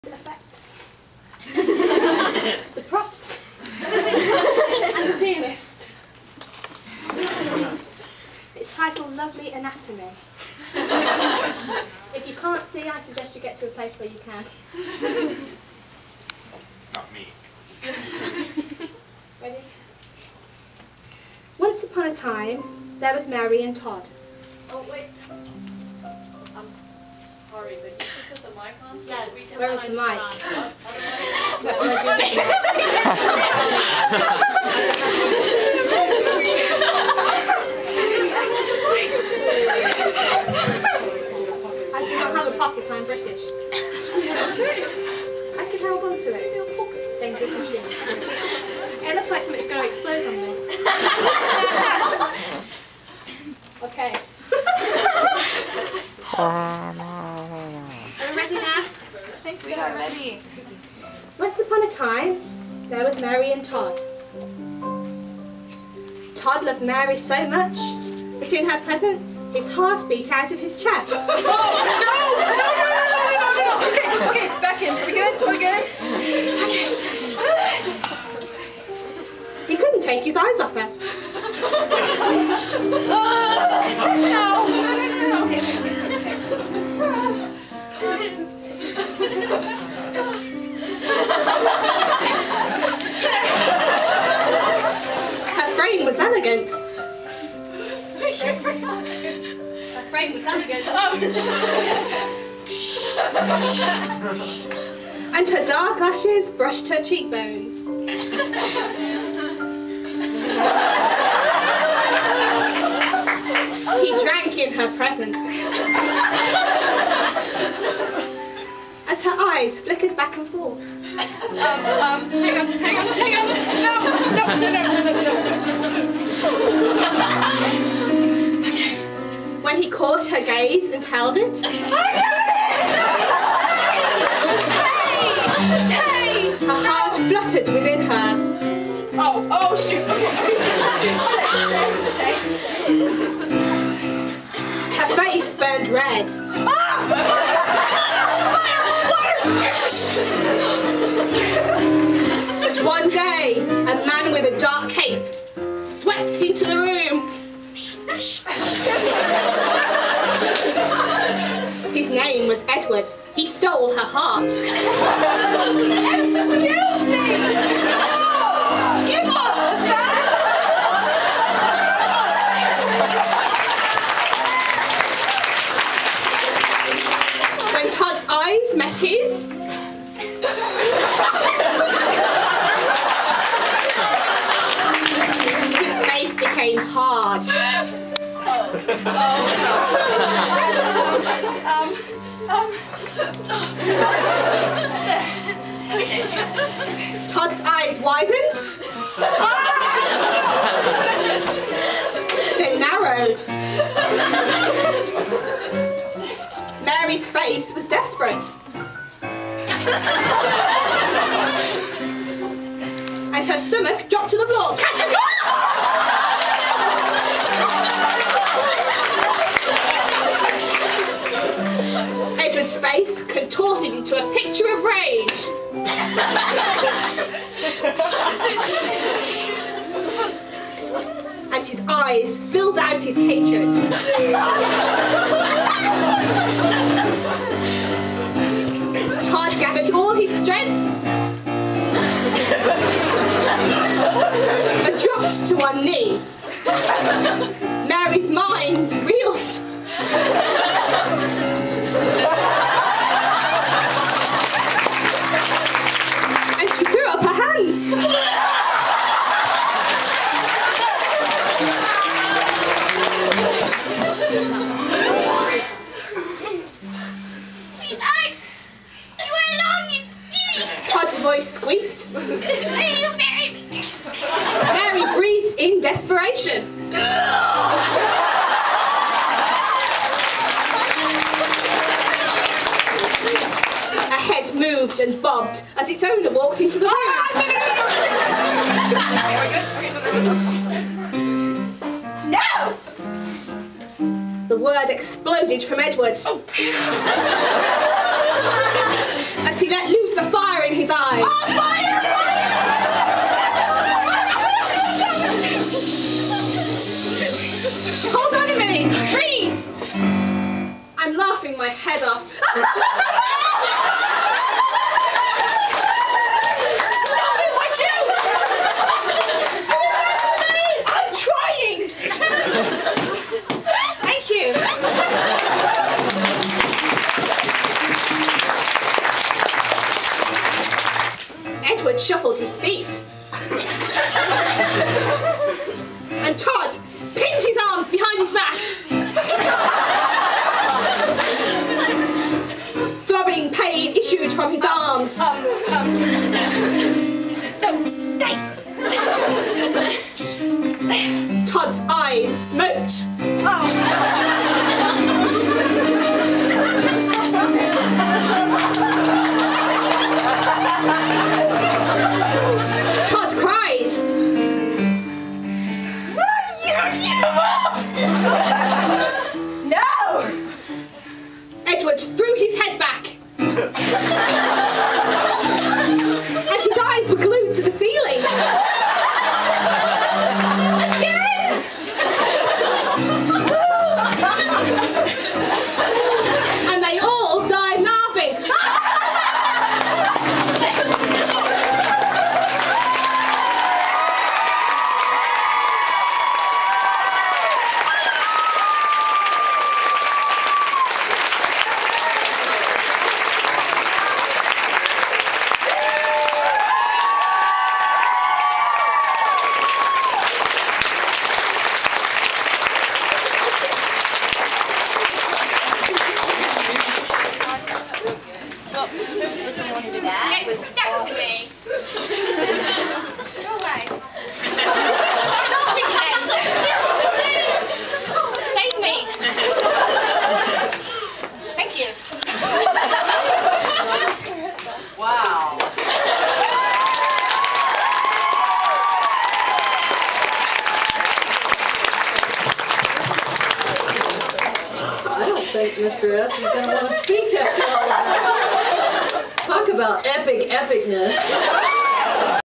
Lovely Anatomy Skit
The final video taken on my camera (though by Daniel Beals, which explains for the strange movement and weird noises at one point) at the OYAN Summer Workshop 2011, was the skit on loose body parts done by the Laughing Lothlorian Ladies (critique group) and dedicated to Bryan Davis.

Credits:
Todd - Storygirl
Mary - God'sGirl95
Edward - BattleMaiden
Sound Effects - RanchGirl and SilverWings
Props - Pippin
Pianist - Matthew "Spellchecker" Lauser/Luke Alistar
Narrator - Jane Johnson/Queen Jane by God's Grace